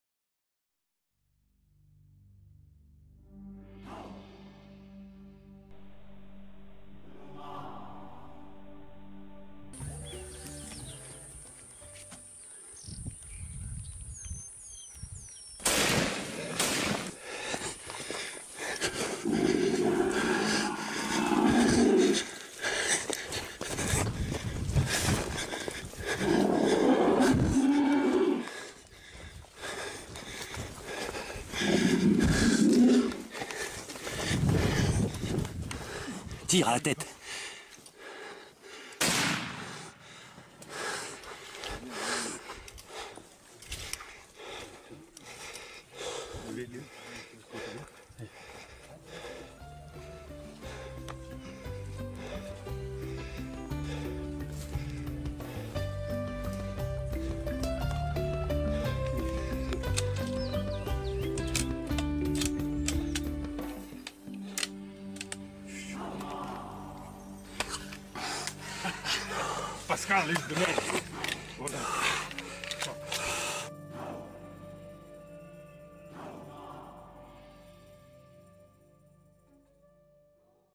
PascalOlmeta - veut faire disparaître une vidéo où il abat un éléphant lors d'un safari au Zimbabwe.
PascalOlmeta, ancien gardien de l'OM, veut faire disparaître une vidéo où il abat un éléphant lors d'un safari au Zimbabwe offert par un fabricant de munitions. Le joueur se plaint d'avoir perdu de l'argent et d'avoir subi une "salve d'injures".

On rappelle que les éléphants sont en voie de disparition. 🐘